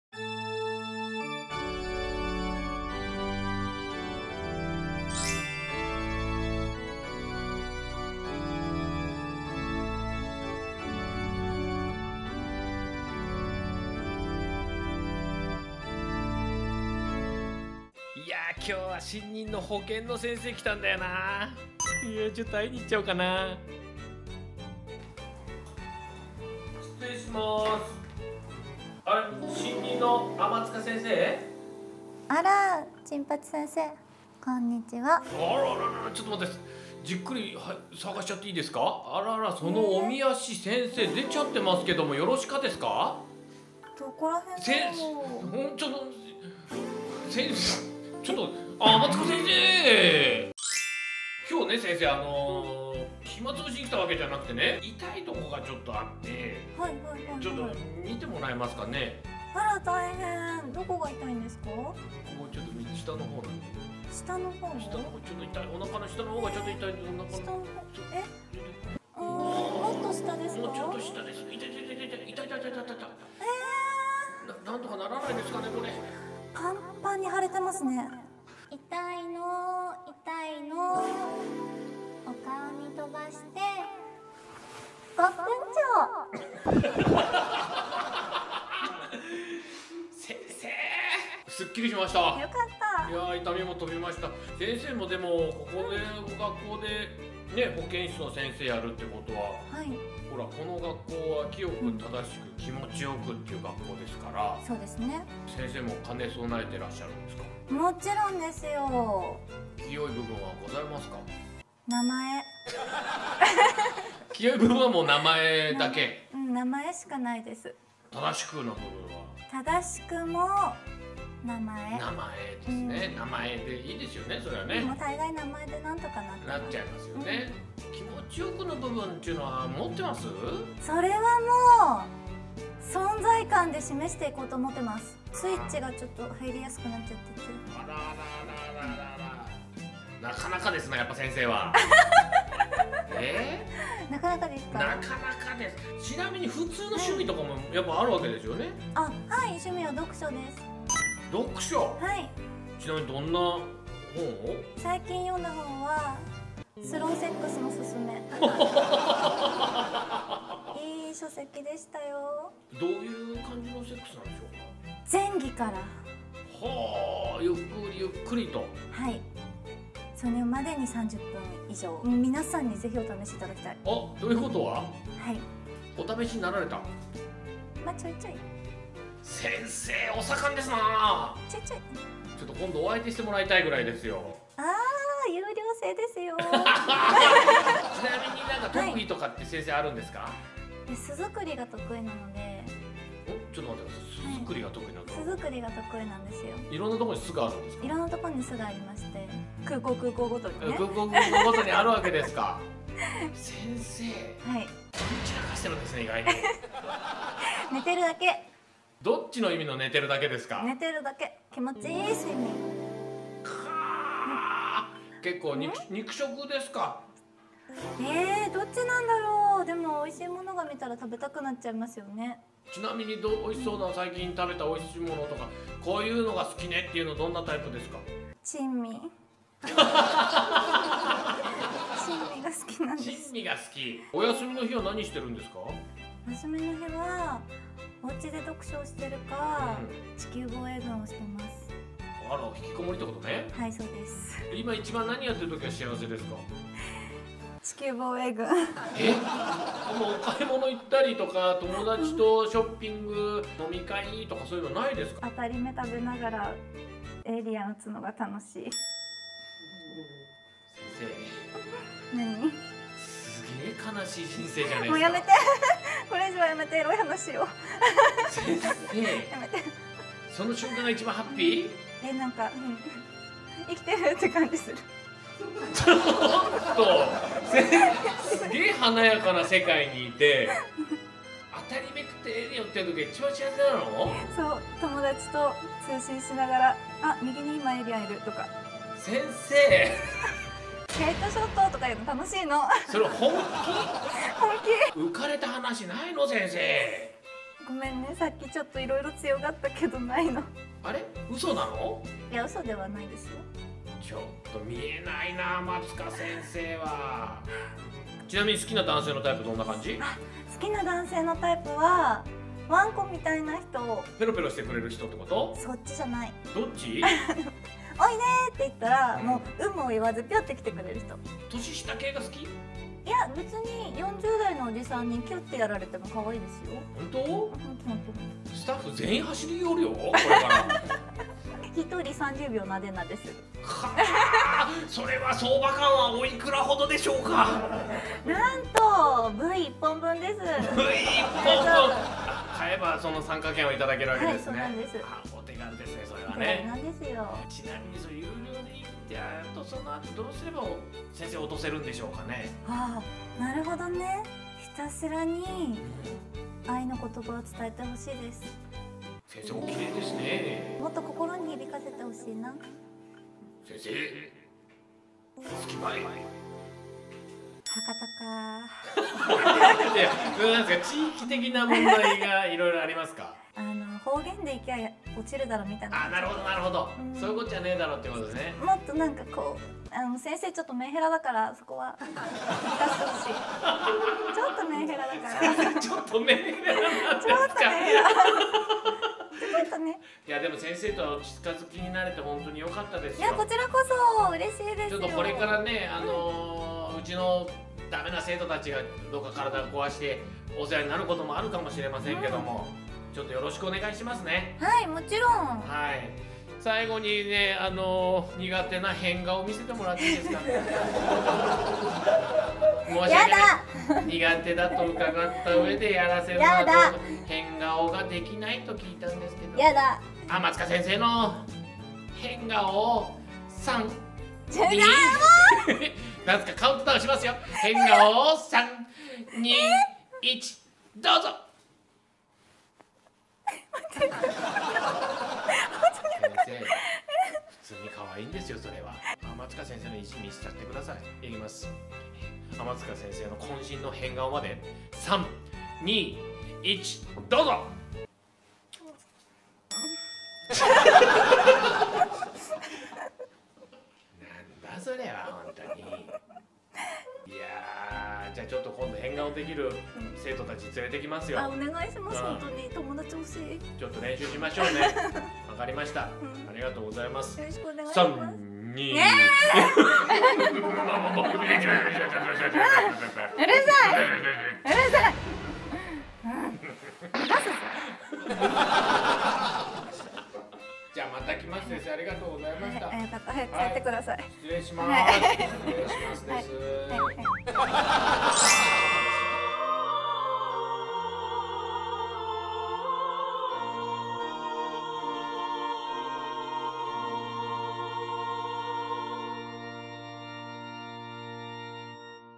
遂に天使降臨！！白衣の天使もえ先生にテンション上がりすぎてチン八先生の無茶振りがエスカレート！？聖ファレノ女学院 #7『保健室の1コマ』〜天使もえ編〜
FALENO専属女優だらけの女子校がついに開校！
今回は入学面接編ではなく、普段の学園生活の1コマの「保健室」に潜入！
白衣の天使と呼ばれることが多い保健の先生と言えば…【天使もえ】しか居ないでしょう！！
こんな先生が居たら仮病使ってでも会いに行っちゃいますよね！

天使もえ先生に対して具合が悪くなったと言い張るチン八先生は、いったいどんなぶっ込みをするのか！？お楽しみください！